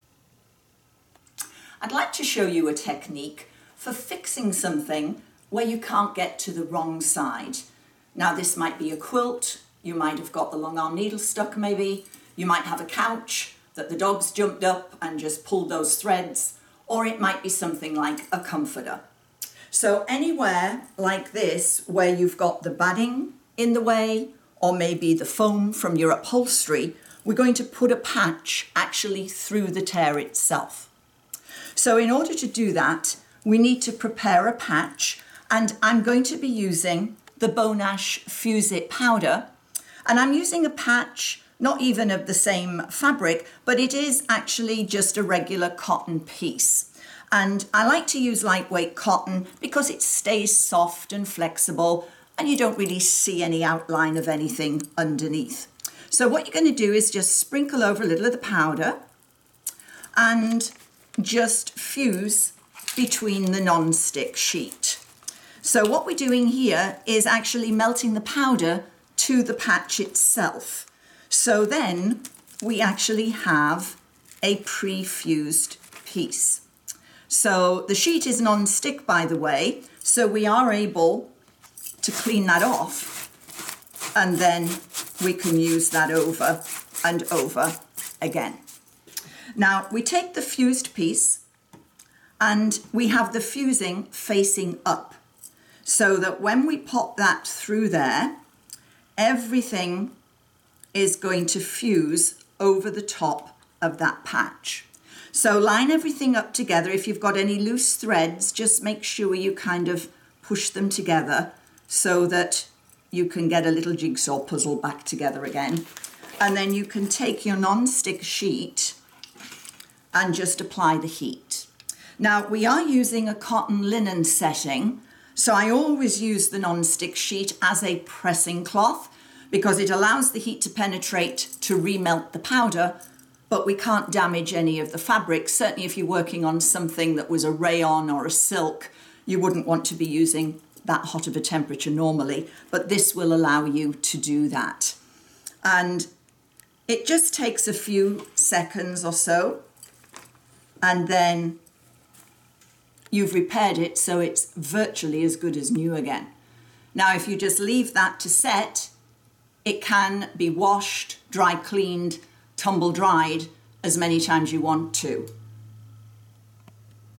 Repair a rip in your quilt! Invisibly
If you slip with your rotary cutter or get your long arm needle stuck.  Do Not Worry, all you need is Fuse It Powder